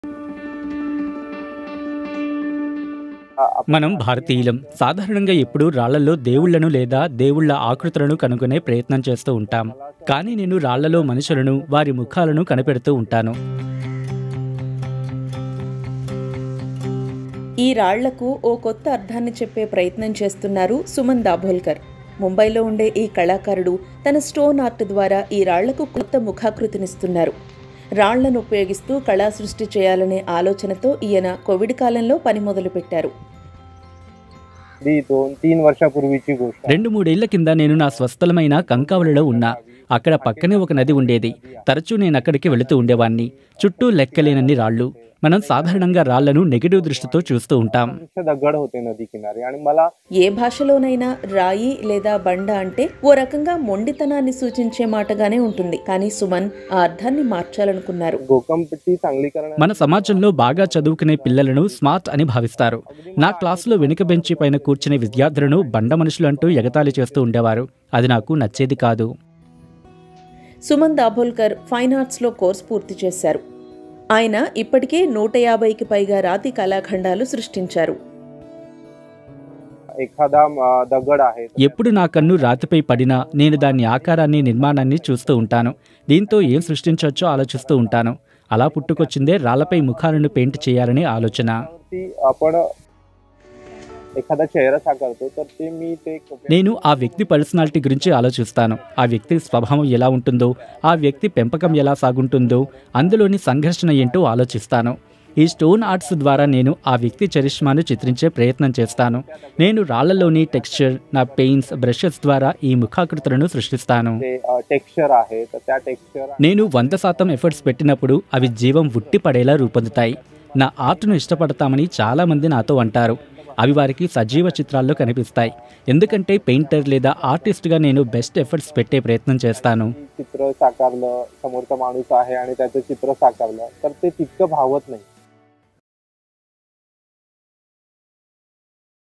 Stone Art: బండరాళ్లపై ప్రముఖుల చిత్రాలు గీస్తూ అబ్బురపరుస్తున్న ముంబై యువ కళాకారుడు | BBC Telugu
కోవిడ్ సమయంలో ఆయన మొదలు పెట్టిన ఈ ప్రయత్నం ఇప్పుడు పలువురిని ఆకర్షిస్తోంది. మొండితనానికి ప్రతిరూపం అయిన రాళ్లు, బండలపై సుమన్ వేసిన చిత్రాలను చూస్తే అద్భుతం అనాల్సిందే. రాయి ఆకారాన్ని బట్టి ఆయన వాటిపై చిత్రాలు గీస్తున్నారు.